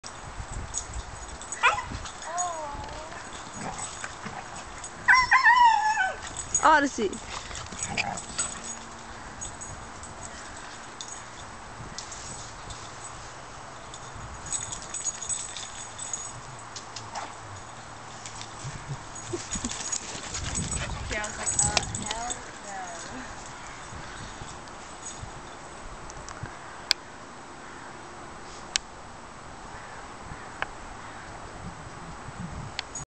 Odyssey & Kiara 3
Odyssey, a Weimaraner puppy, and Kiara, a Native American Indian Dog puppy, playing together.